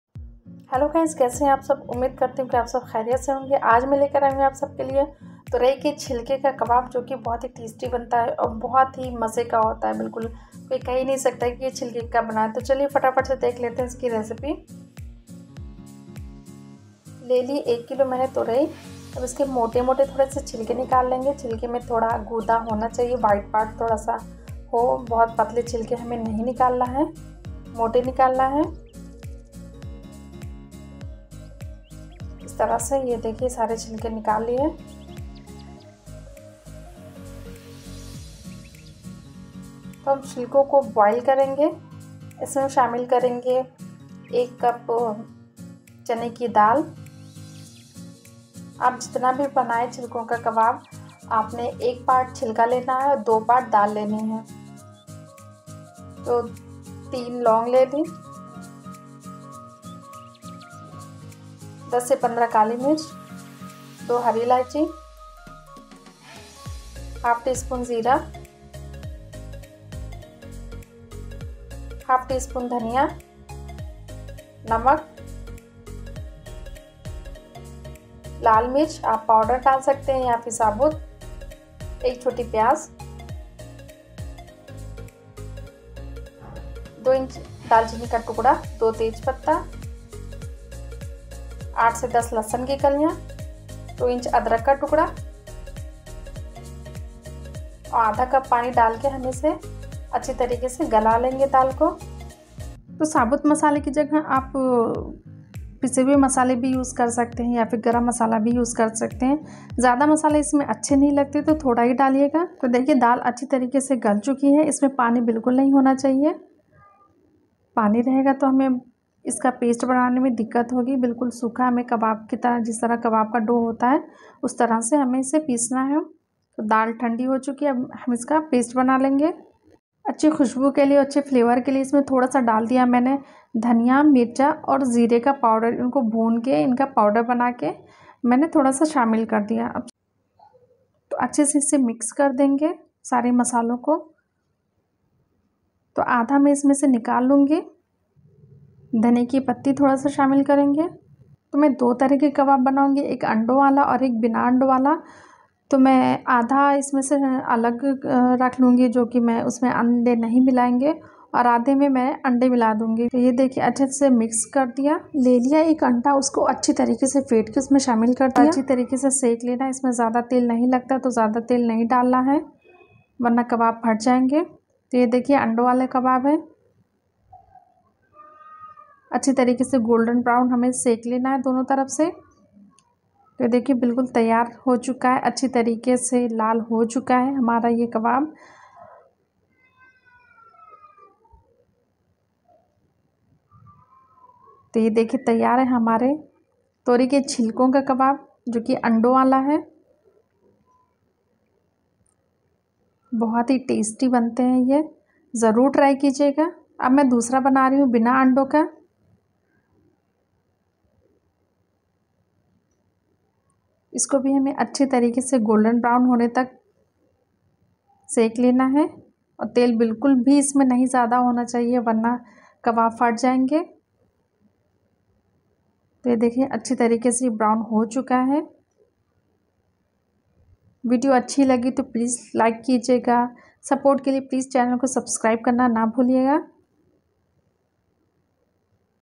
Turai ke chilke ka Kabab very easy and tasty 😋bling with shahin
Hello guys Aaj Mein Lekar I hun aap Sabke Liye Tori ke chhilke ka Kabab to Chalo banate hain

ingredient
1- 1kg  Torai
2 - 1 cup  chana dal
3- e cloves
4- 10,15 black paper
5- 2 green cardamom
6- ½ tsp cumin seeds
7- ½tsp coriander seeds
8- salt
9-red chilli
10- 1 onion
11- 2 inch cinnamon sticks
12- 2 bay leaves
13- 8,10 garlic cloves
14- 2 inch Ginger
15 ½ cup of water
16- 1 egg
17 ¼ spoon roasted cumin, coriander, red chili powder

khushbudar bhuna Masala bling with shahin recipe in hindi